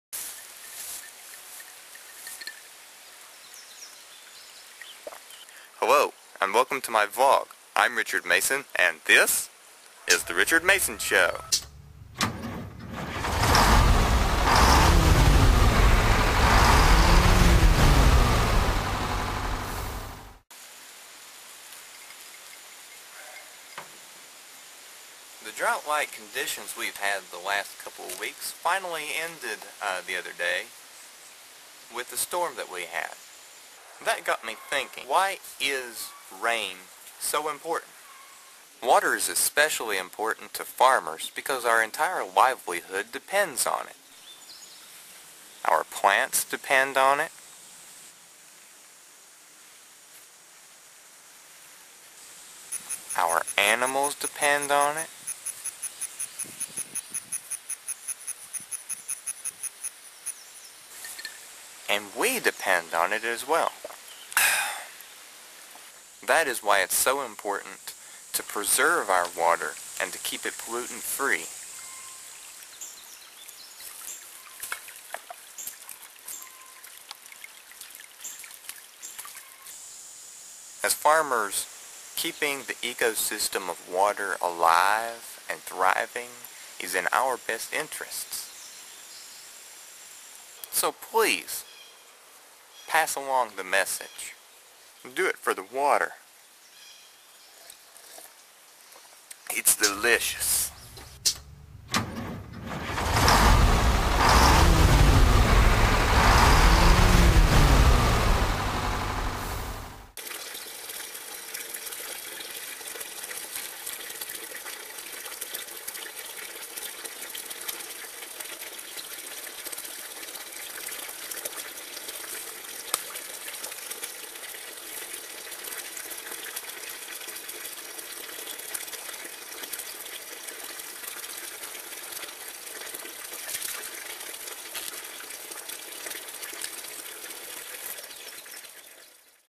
Water!!! | The Richard Mason Show
I contemplate the importance of rain and water.